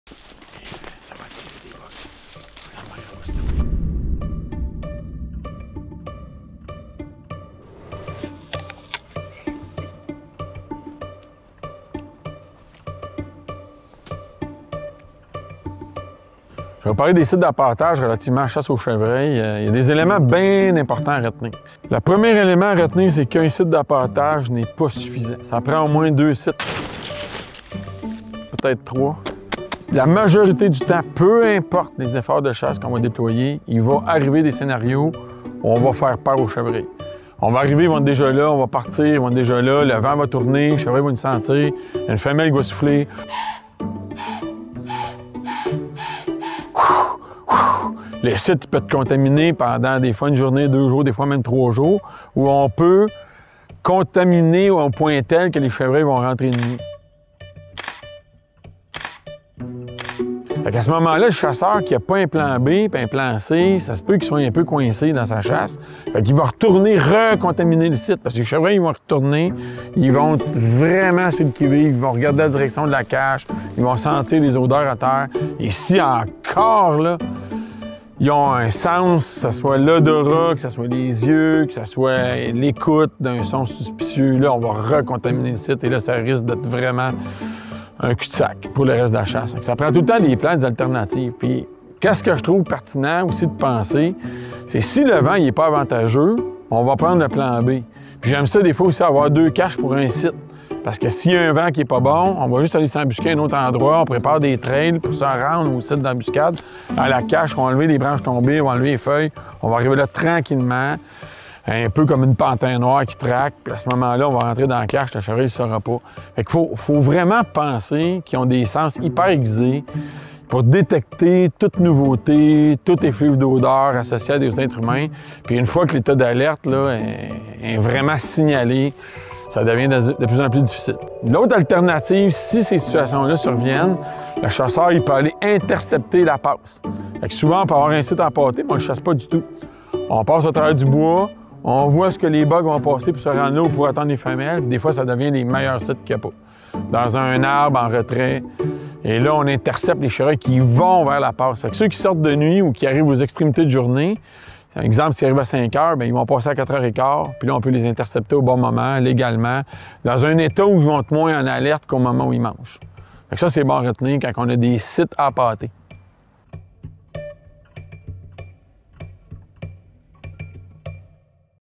Chevreuil appâter de façon rusée
Comment gérer vos sites appâter en fonction de maximiser vos efforts et votre temps de chasse.

Les sites d'appâtage sont une méthode efficace de concentrer les chevreuils à un même endroit. Il peut s'agir d'une bénédiction comme d'une malédiction puisque, lorsque les cerfs détectent votre présence, ils deviennent suspicieux du site.